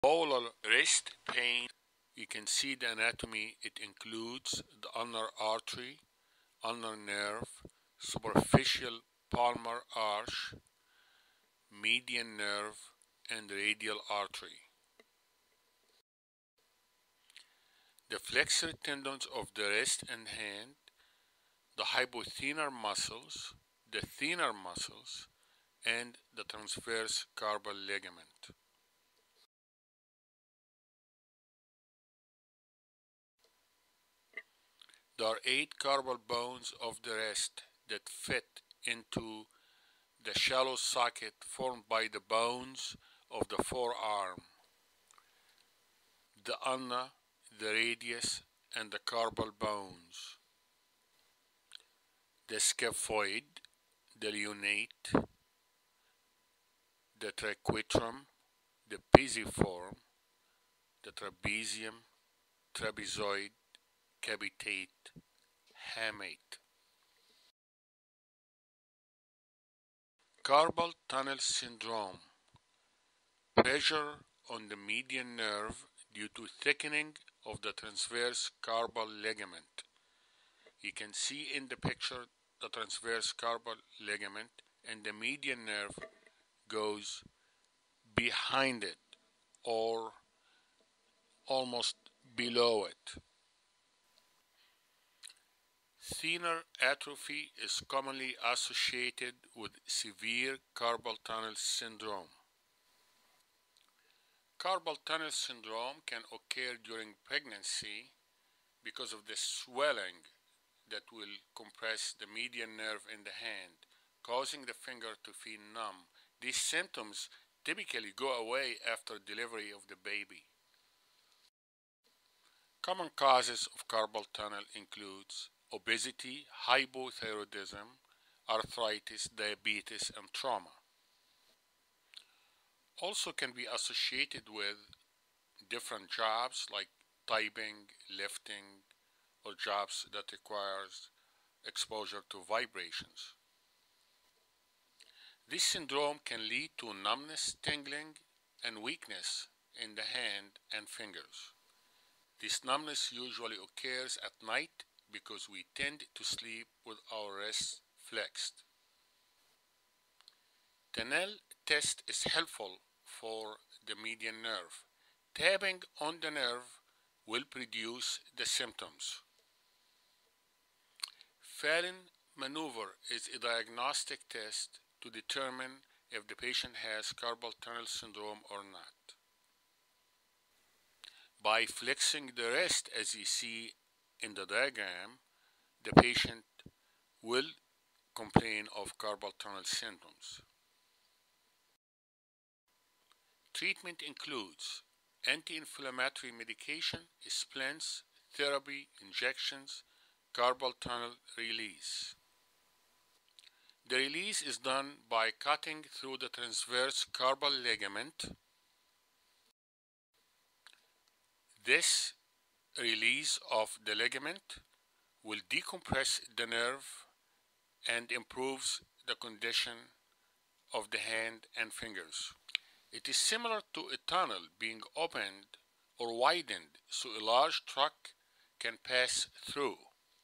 wrist pain, causes and treatment
there are eight carpal bones of the wrist that fit into a shallow socket formed by the bones of the forearm.
Pressure placed on the median nerve due to thickening of the transverse carpal ligament.
Thenar atrophy is commonly associated with severe carpal tunnel syndrome.
Carpal tunnel syndrome can occur during pregnancy since the swelling brought on by pregnancy may compress the median nerve in the hand, causing the fingers to feel numb. These symptoms typically go away after delivery of the baby.
Common causes of CTS include:
•Obesity
•Hypothyroidism
•Arthritis
•Diabetes
•Trauma
•Repetitive work (typing, lifting)
•Vibration
Carpal tunnel syndrome can lead to numbness, tingling or weakness in the hand and fingers. The numbness usually occurs at night because we tend to sleep with our wrists flexed.
The Tinel’s test is used to determine symptoms of carpal tunnel syndrome. The test is considered positive if symptoms of tingling worsen while tapping on the median nerve at the wrist.
The Phalen's maneuver is a diagnostic test performed to determine if the patient has carpal tunnel syndrome. The back of the hands are pressed together, compressing the nerve, which may cause symptoms of carpal tunnel syndrome.
Treatment includes
•Anti-inflammatory medication
•Splint
•Injections
•Carpal tunnel release:
performed by cutting through the transverse carpal ligament.
Carpal tunnel release allows the median nerve to pass freely through the tunnel to receive sensations from the thumb, index, and middle fingers of the hand. This is similar to a tunnel being opened or widened so a large truck can pass through.
special thanks for NATHAN ELKINS FOR HIS CONTRIBUTION TO THIS VIDEO